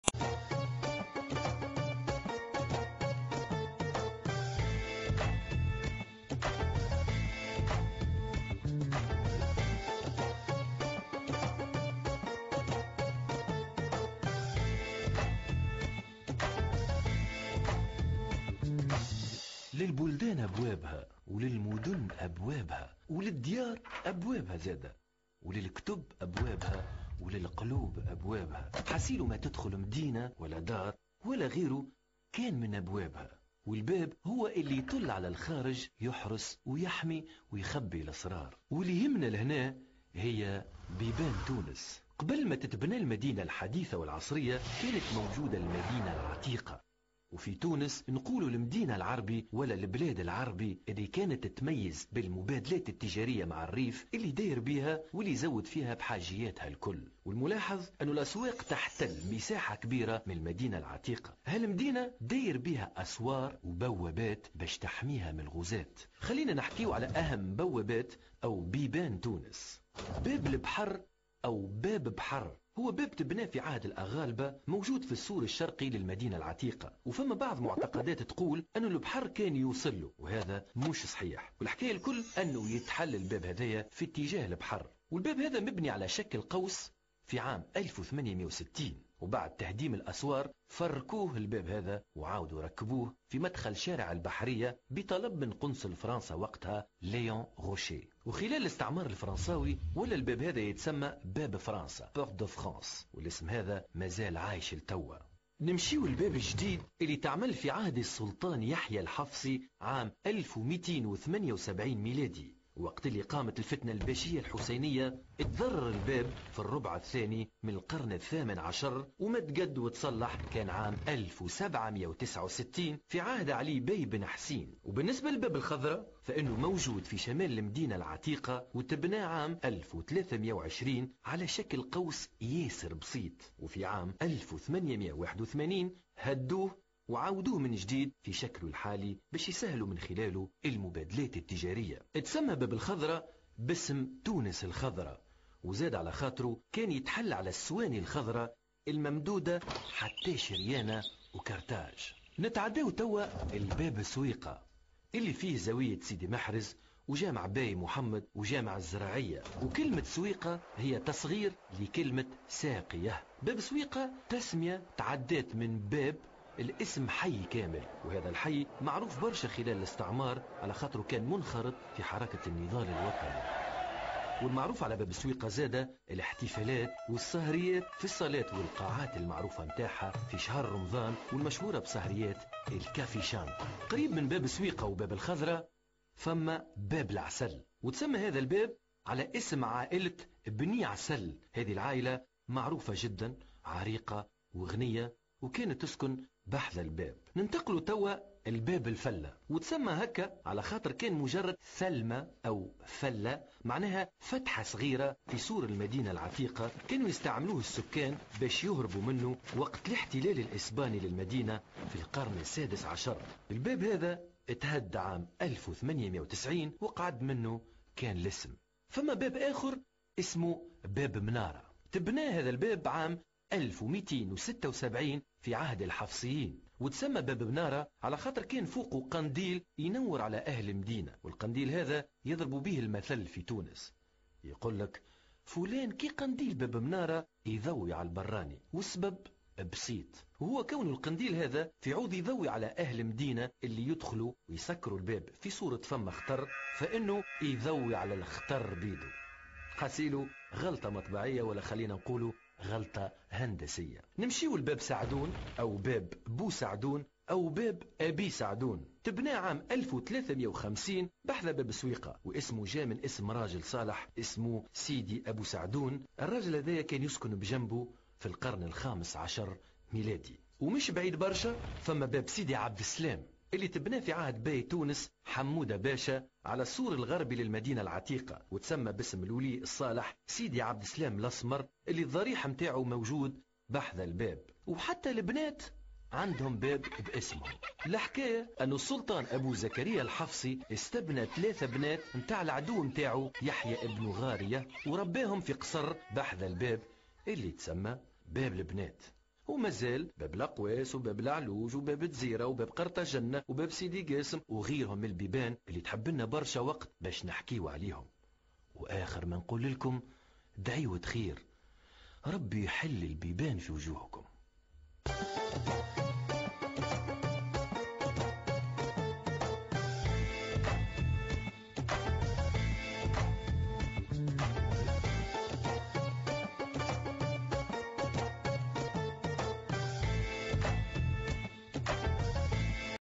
les portes de Tunis.mp4
Dès sa fondation, Tunis est considérée comme une importante base militaire. elle était entourée d'un mur (enceinte intérieure) parsemé de différentes portes (7 ou 8 donnée à creuser encore) : Bab El Jazira, Bab Carthagena, Bab Souika , Bab Menara & Bab El Bhar. ces portes, permettaient l'accès à la médina, les faubourgs et les principales routes de commerces et de ravitaillements.

Au début du règne des Hafsides, deux nouvelles portes sont percées au XIIIe siècle : Bab Bnet & Bab Jedid.
Avec le développement de la capitale, deux faubourgs émergent à l'extérieur des remparts : Bab El Jazira (au sud) et Bab Souika (au nord). C'est pourquoi, le souverain hafside Abû Darba Muhammad al-Mustansir al-Lihyânî ordonne, au début du XIVe siècle, la construction d'une seconde enceinte englobant la médina et ses deux faubourgs extérieurs. Elle est dotée de six portes : Bab El Khadra, Bab Saadoun, Bab El Allouj , Bab Khalid ou Bab Sidi Abdallah Chérif, Bab El Fellah et Bab Alioua.
À l'époque ottomane, quatre nouvelles portes sont ouvertes : Bab Laassal, Bab Sidi Abdesselam, Bab El Gorjani et Bab Sidi Kacem